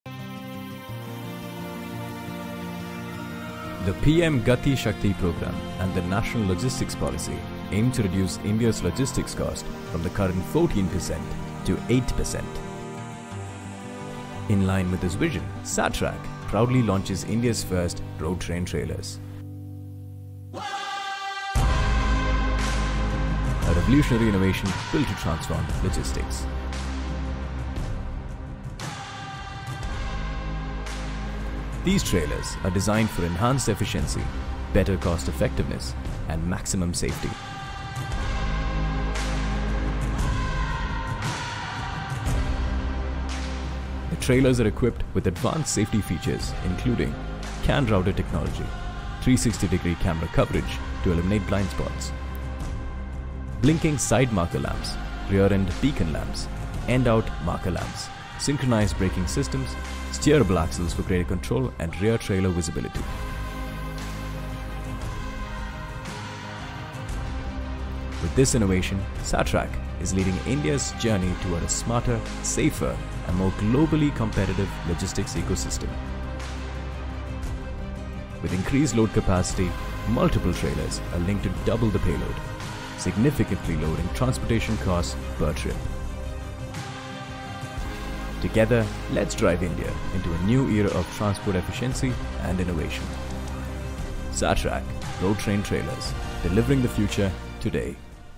SATRAC Redefines Long-Haul Logistics | India’s First Road Train | India’s first double-trailer
SATRAC has launched India’s first Road Train Trailers, a game-changing innovation set to transform the logistics industry. With unmatched payload capacity, cutting-edge safety tech, and next-level efficiency, this is the future of freight transport in India.
From CAN Router technology for instant communication to 360° camera coverage, steerable axles, and synchronized braking systems, SATRAC’s Road Train is built to meet India’s logistics reform goals under PM Gati Shakti and the National Logistics Policy.
Key Highlights:
First-ever Road Train Trailers in India
Higher payloads, fewer trips, lower costs
Advanced safety with full visibility and control
Designed for India’s dynamic roads & logistics needs
Whether you’re a fleet owner, logistics professional, or trucking enthusiast, this is one innovation you can’t miss.